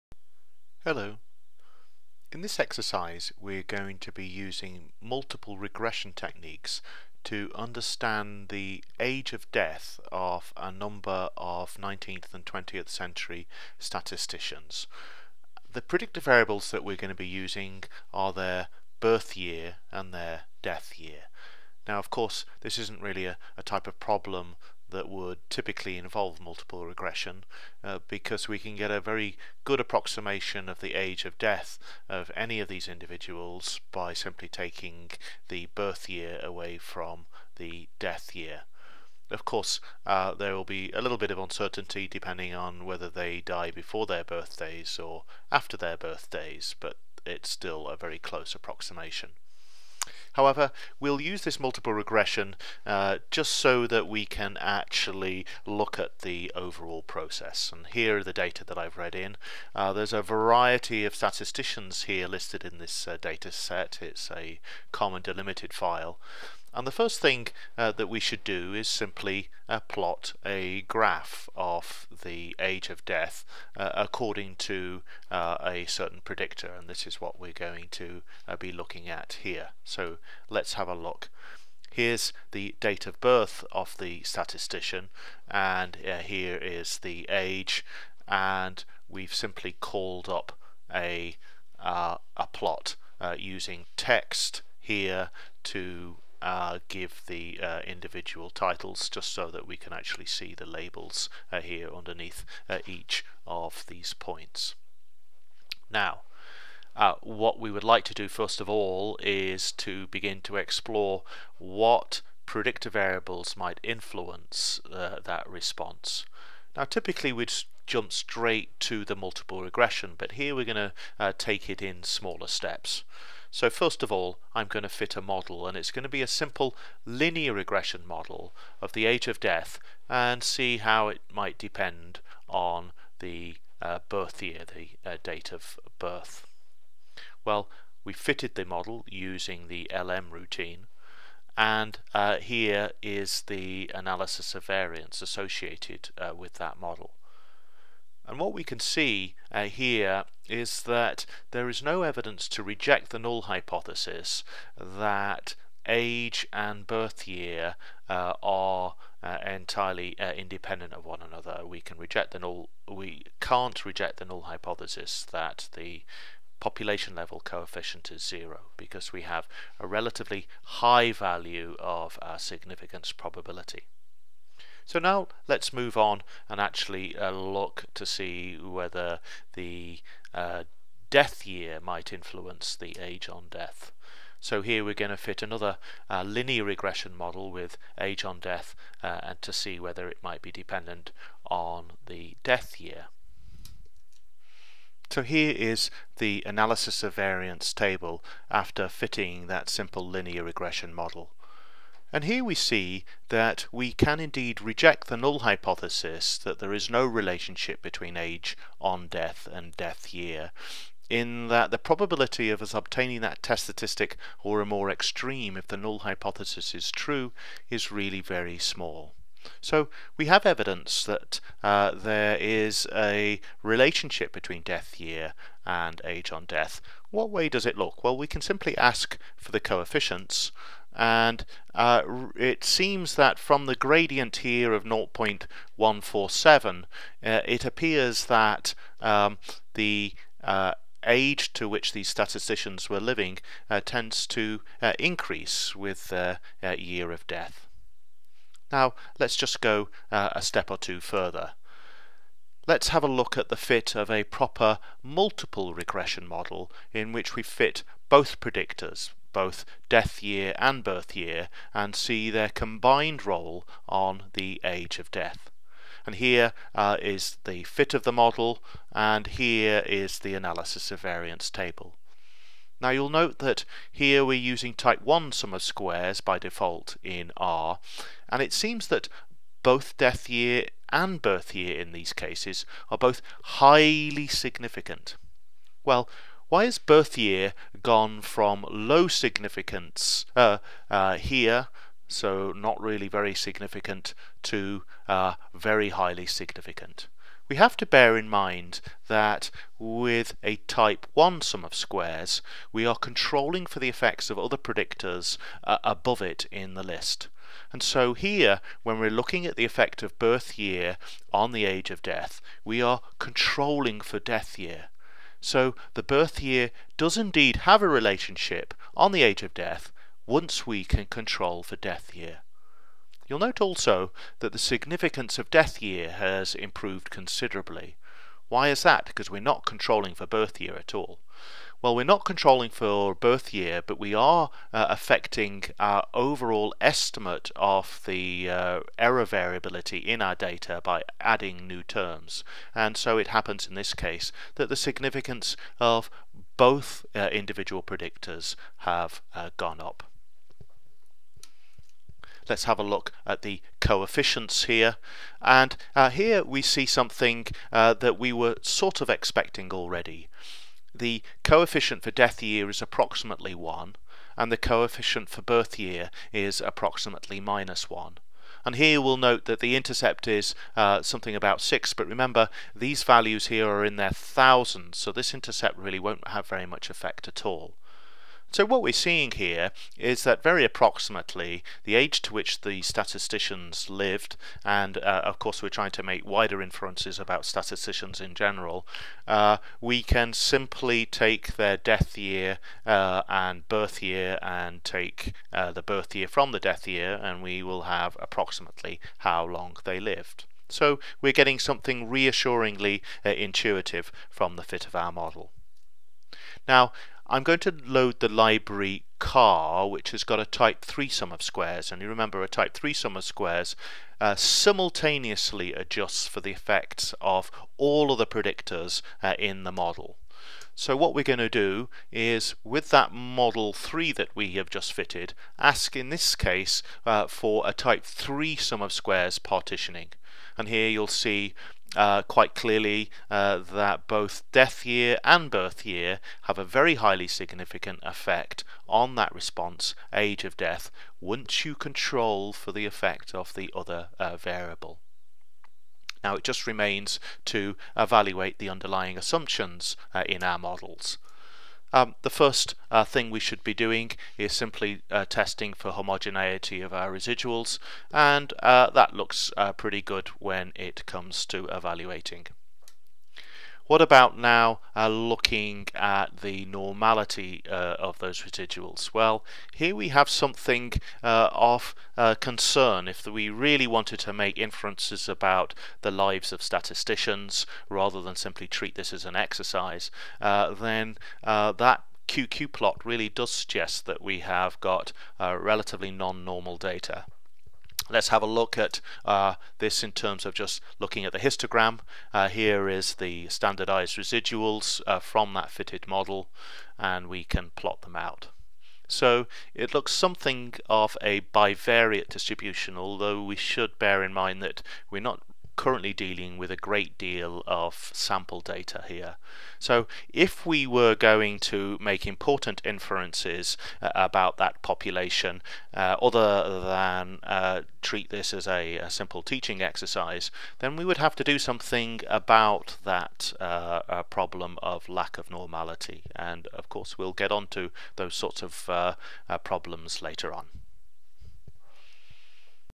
Multiple Regression in R
An introduction to Multiple Regression in R using RStudio.